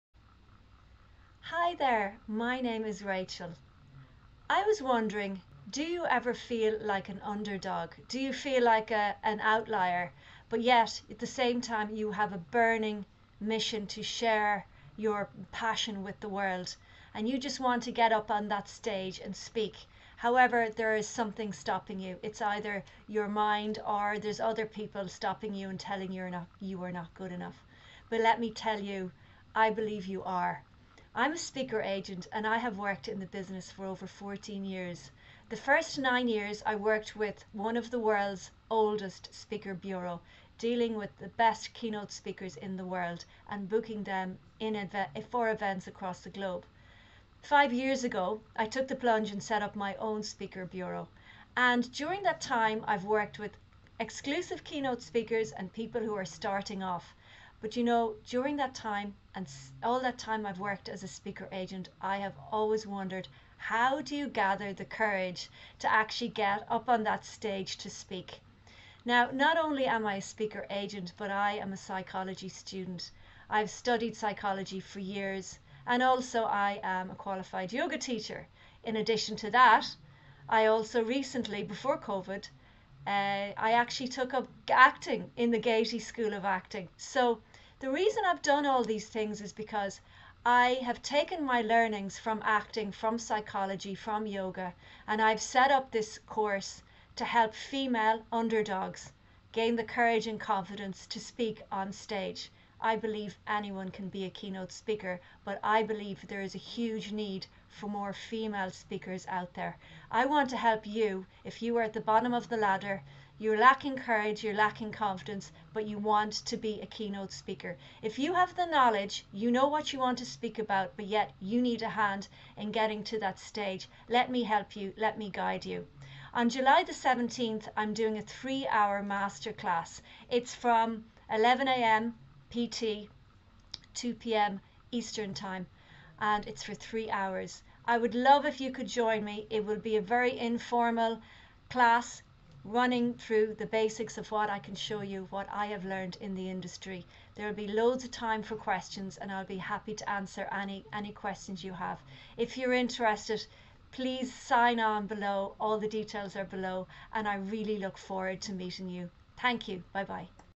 The Female Underdog Speaker Programme
Are you a female Outlier?
Do you feel like an Underdog?
Do you have a burning passion to share with the world?
Are you lacking in confidence, courage and business KNOW-HOW on even HOW to SPEAK UP and get on that Stage?
If so, I can help you
On Saturday July the 17th at 11:00 AM PT / 14:00 PT I will be hosting a Masterclass for three hours to help you.
For more details please go to mastermind.com

This is a promotional video for my mastermind class taking place on Saturday the 17th of July